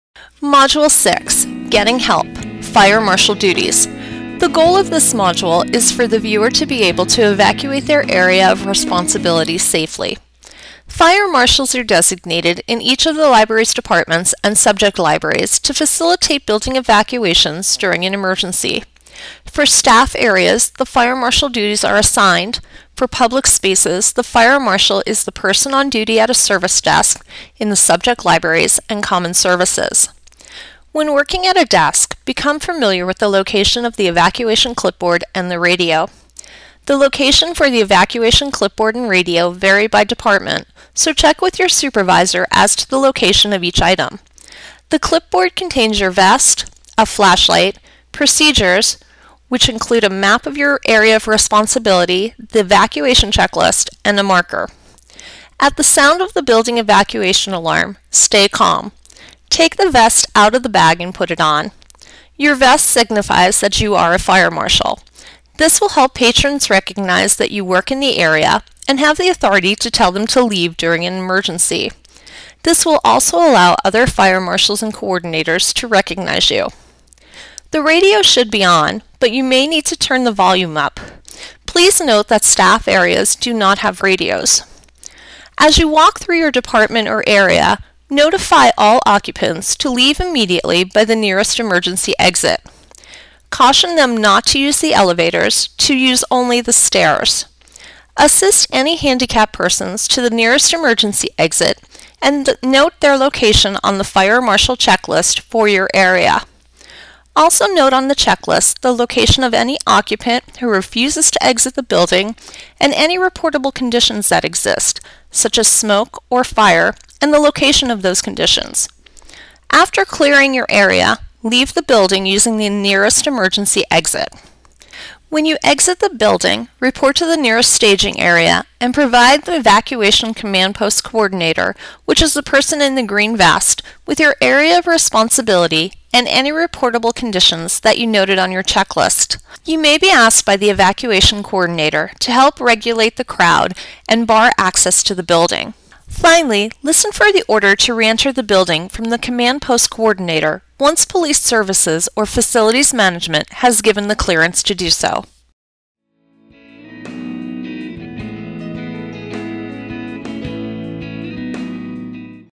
Module 6 - Getting Help - Fire Marshal Duties
The goal of the module is for the viewer to be able to evacuate their area of responsibility safely